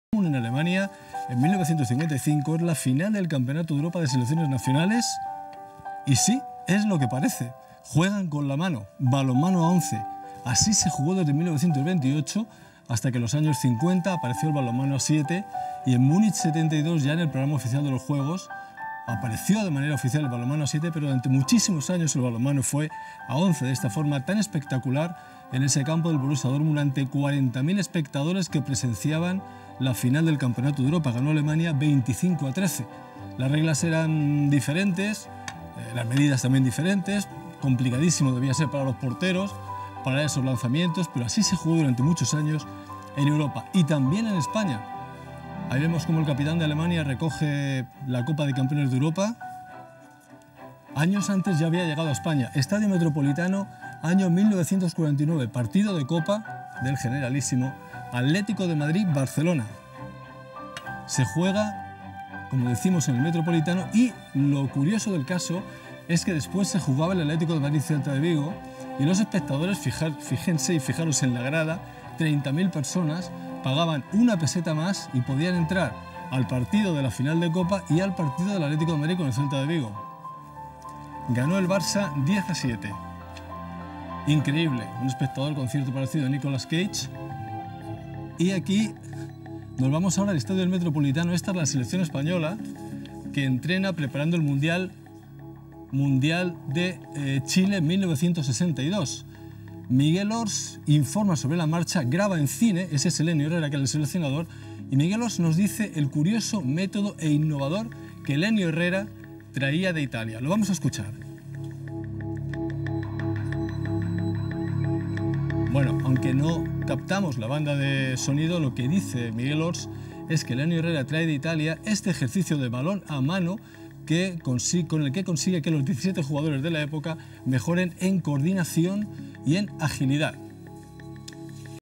Cuando el balonmano se jugaba en campos de fútbol
En los inicios del balonmano se jugaba en campos de fútbol y once contra once. Las reglas fueron cambiando, pero hubo un tiempo en que fútbol y balonmano estaban unidos.